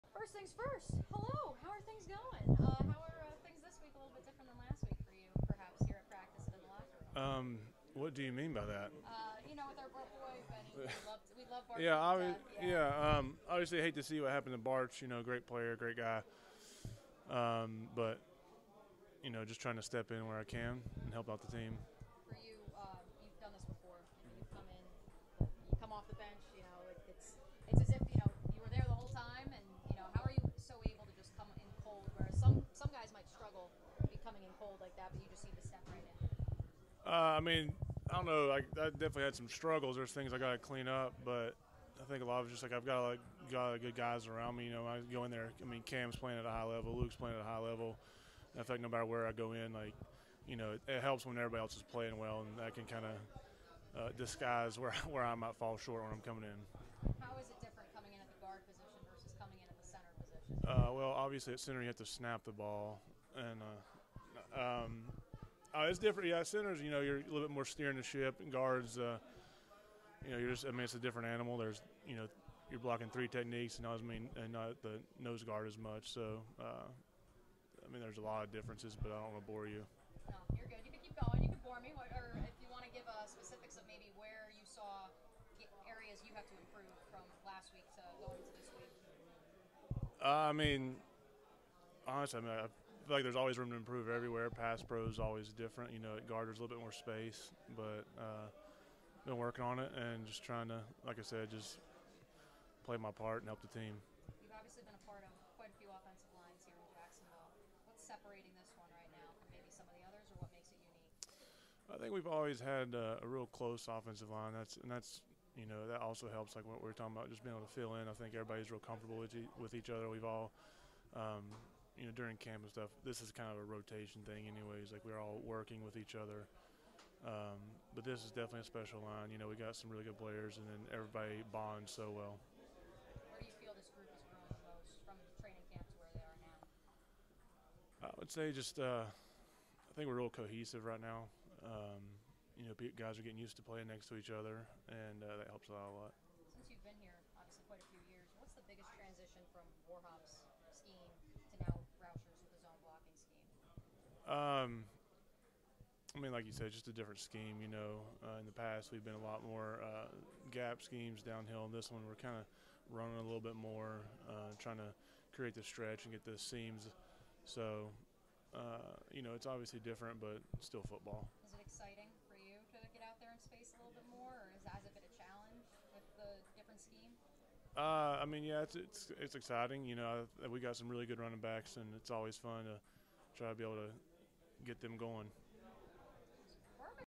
WATCH: Tyler Shatley on Jaguars’ preparing to face the Colts
Offensive lineman Tyler Shatley discusses the Jaguars’ upcoming Week 6 matchup with the Indianapolis Colts.